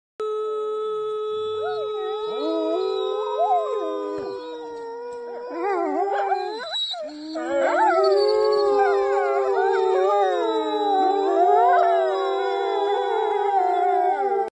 Howling with the wolves 🐺 In Norway Polar Park
Howling with the wolves! 🐺 In Norway's Polar Park, the world's northernmost animal park that places great emphasis on animal welfare, you can learn more about these beautiful, majestic animals that are often misunderstood. 💡

Here's Anneka Svenska, host of Animal Watch, being accepted into the wolf pack as she howls alongside them. 😮 The animal sanctuary aims to educate both visitors about Arctic animals and the importance of preserving natural heritage. ✨

Have you gotten up close and personal with wolves?

📍 @polarpark, Norway